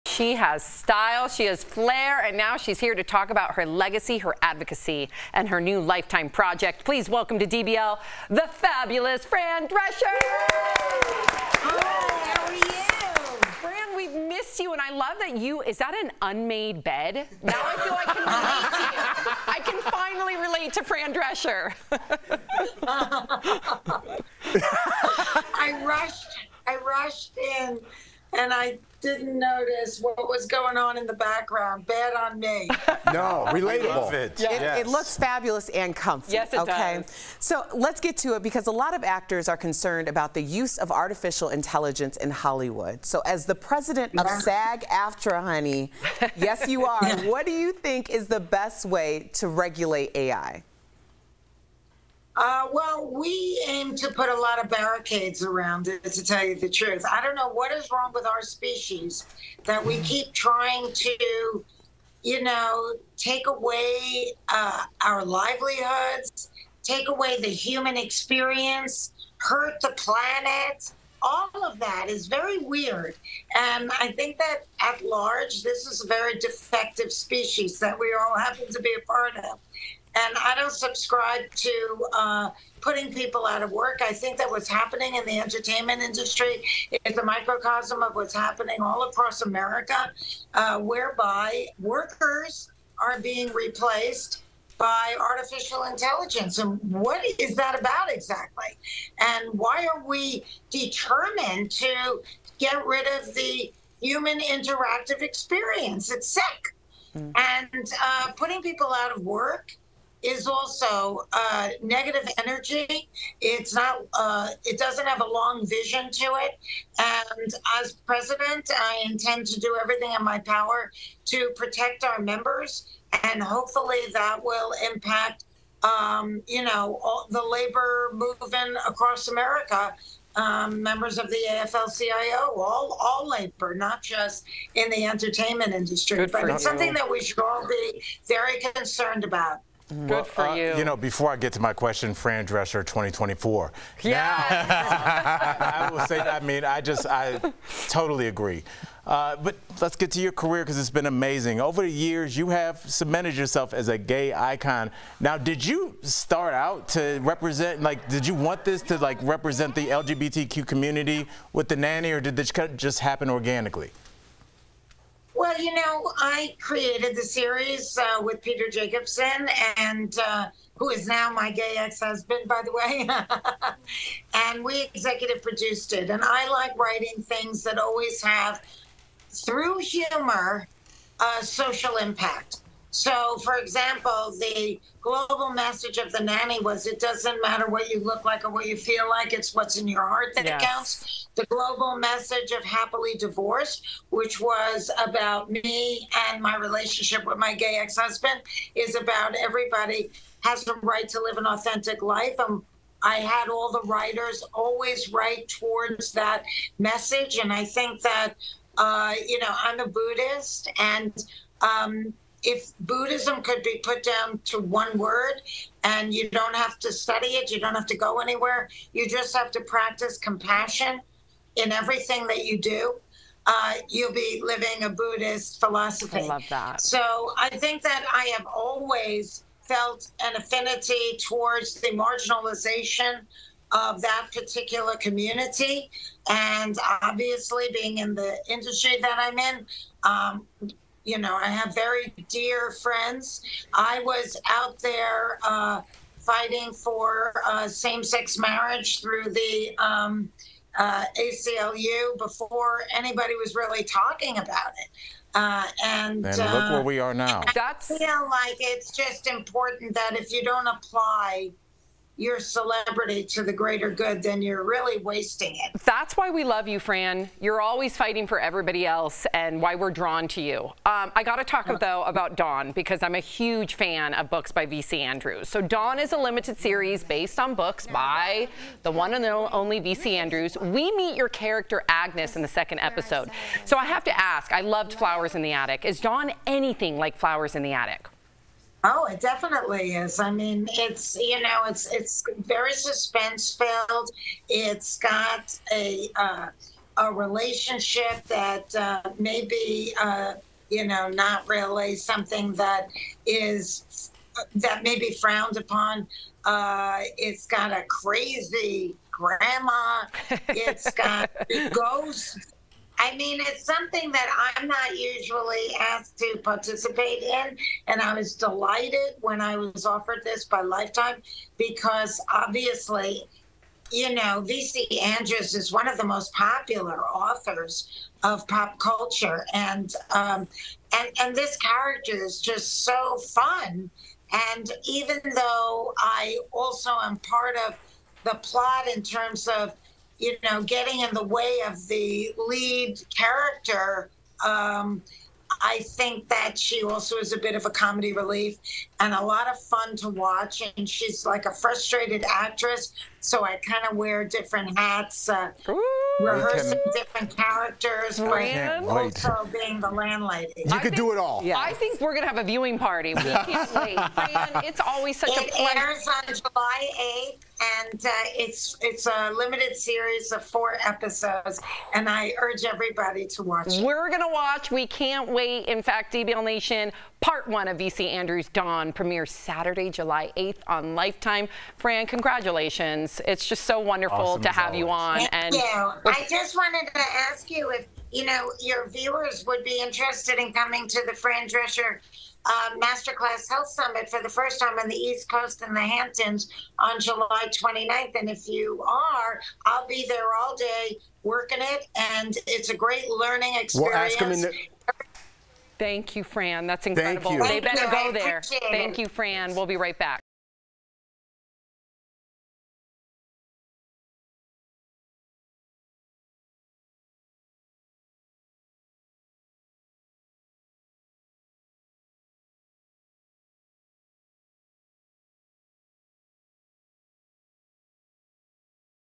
Fran Drescher Talks A.I. in Hollywood, Being a Gay Icon & Lifetime's 'Dawn' Limited Series
Fran Drescher has style, she's got flair, and she joined DBL to talk about her legacy as 'The Nanny,' her advocacy as a gay icon and as the president of the SAG-AFTRA labor union, and her new project playing 'Agnes' in Lifetime's limited series 'Dawn,' which is based on books by author VC Andrews.

 12:30 p.m. - 1 p.m. ET
   2:00 p.m. - 6 p.m. ET
   7:30 p.m. - 8 p.m. ET

🔴 DailyBlastLive.com